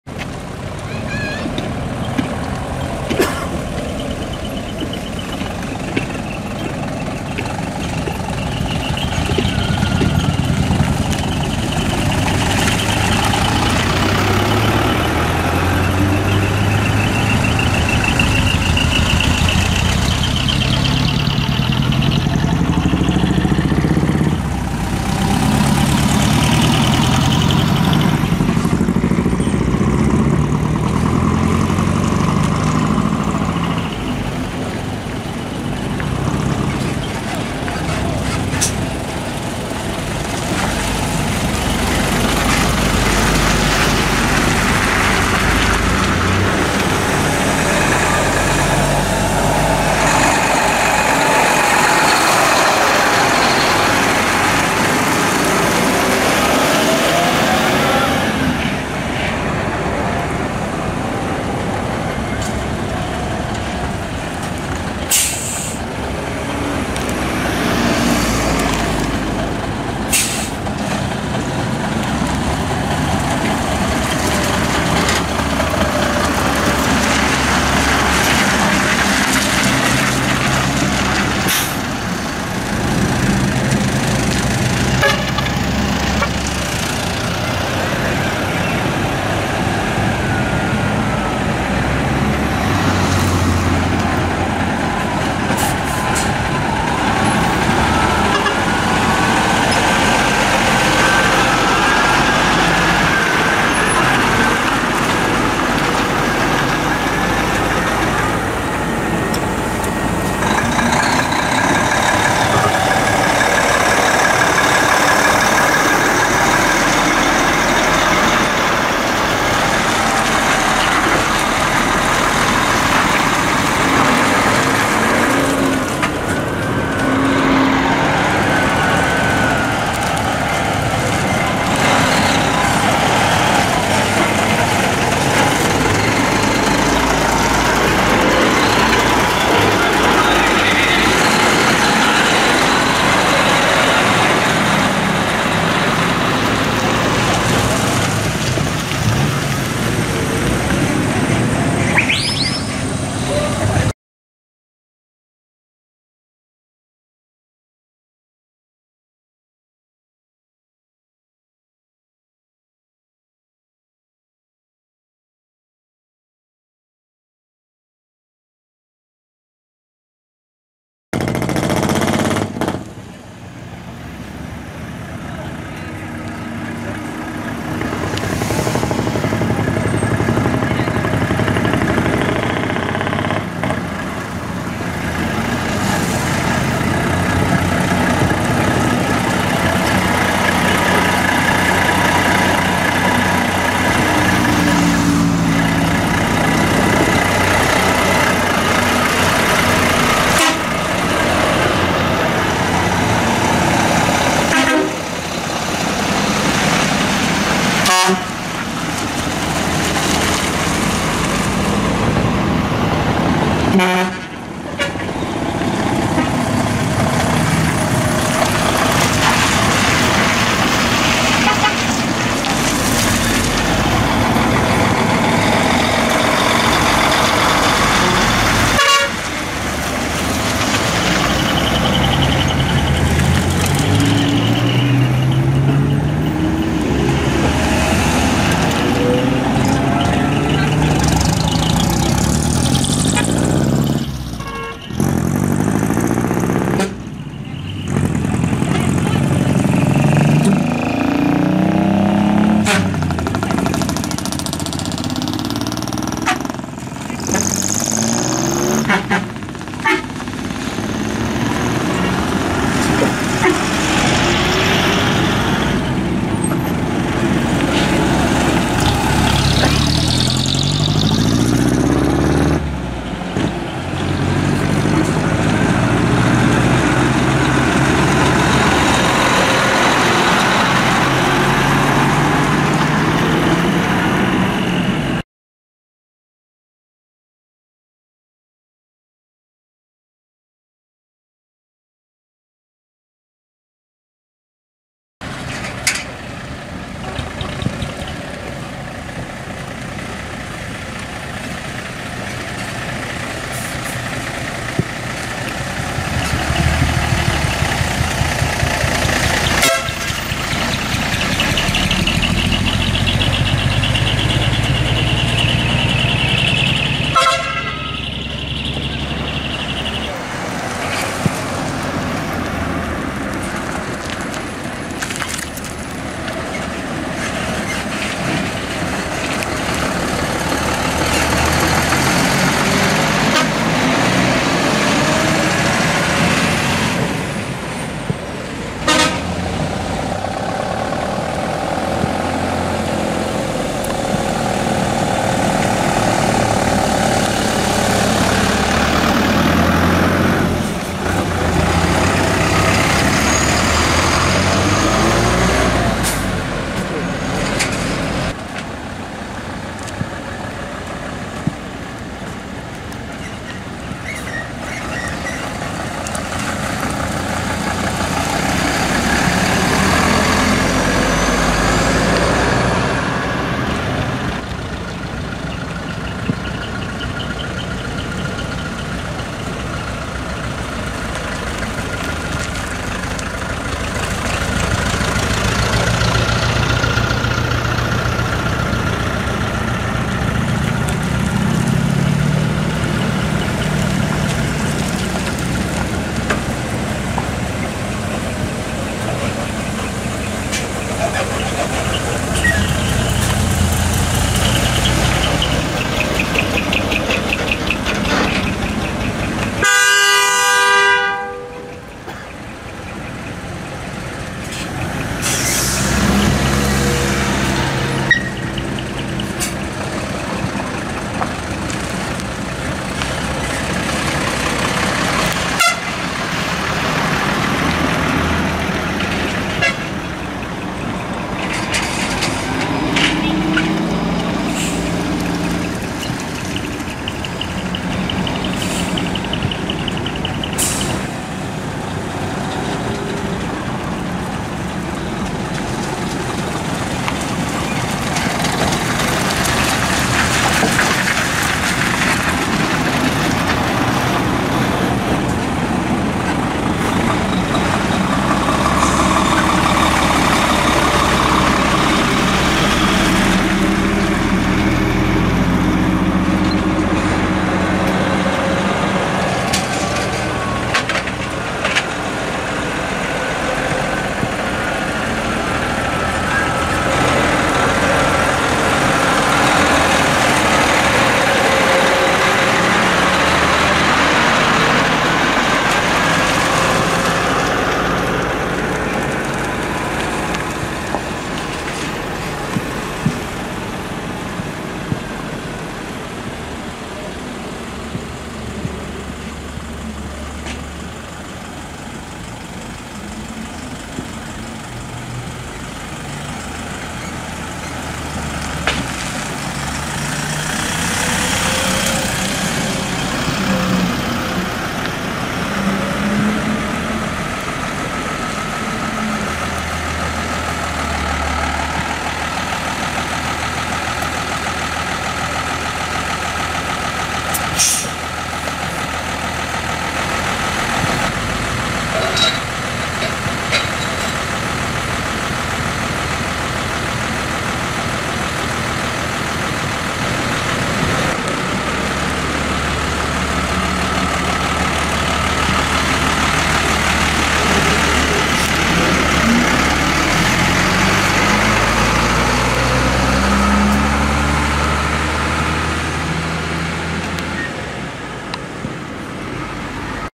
2019雅会 西日本災害復興チャリティ撮影会20
2019/11/10 岡山県浅口市寄島町の三ツ山スポーツ公園多目的広場で開催されたみやび会西日本災害復興チャリティ撮影会の模様です。

第一部 午前7時30分ごろ～午後１時ごろ､第二部 午後4時ごろ～ナイト全車両退出までに出入りのあった車両を中心に撮影しています。全ての車両は撮影できていません。